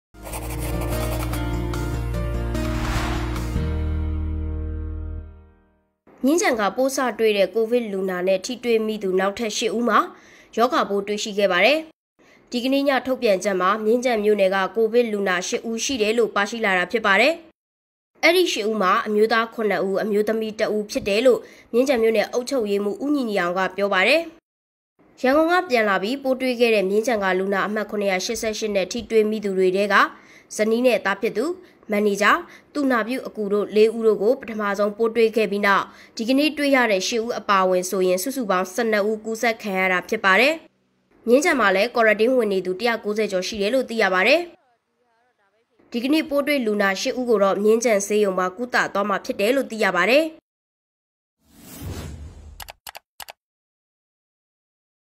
မြင်းခြံမှာ ကိုဗစ်လူနာ ၈ ဦးထပ်တွေ့၊ လူနာ အမှတ် ၇၈၈ ကနေ ကူးစက်သူ ၁၂ ဦးအထိရှိလာ
မြင်းခြံက ပိုးစတွေ့တဲ့ကိုဗစ်လူနာနဲ့ ထိတွေ့မိသူနောက်ထပ် ၈ ဦးမှာ ရောဂါပိုးတွေ့ရှိခဲ့ပါတယ်။

ဒီကနေ့ ည ထုတ်ပြန်ချက်မှာ မြင်းခြံမြို့နယ်က ကိုဗစ်လူနာ ၈ ဦးရှိတယ်လို့ ပါရှိလာတာဖြစ်ပါတယ်။

အဲဒီ ၈ ဦးမှာ အမျိုးသား ၇ ဦး၊ အမျိုးသမီးတစ်ဦးဖြစ်တယ်လို့ မြင်းခြံမြို့နယ်အုပ်ချုပ်ရေးမှူး ဦးညီညီအောင်ကပြောပါတယ်။

ရန်ကုန်က ပြန်လာပြီး ပိုးတွေ့ခဲ့တဲ့
မြင်းခြံက လူနာ အမှတ် ၇၈၈ နဲ့ထိတွေ့မိသူတွေထဲက ဇနီးနဲ့သားဖြစ်သူ၊ မန်နေဂျာ၊ သူနာပြုအကူတို့ လေးဦးတို့ကို ပထမဆုံးပိုးတွေခဲ့ပြီးတဲ့နောက် ဒီကနေ့ တွေ့ရတဲ့ ၈ ဦးပါအဝင်ဆိုရင် စုစုပေါင်း ၁၂ ဦး ကူးစက်ခံရတာဖြစ်ပါတယ်။

မြင်းခြံဒီဂရီကောလိပ်မှာလည်း ကွာရာတင်းဝင်နေသူ ၁၉၀ ကျော်ရှိတယ်လို့သိရပါတယ်။

ဒီကနေ့ ပိုးတွေ့လူနာ ၈ ဦးကိုတော့ မြင်းခြံဆေးရုံမှာ ကုသသွားမှာဖြစ်တယ်လို့ သိရပါတယ်။

ရုပ်သံသတင်းထောက် - ဖြိုးဖြိုးဇော်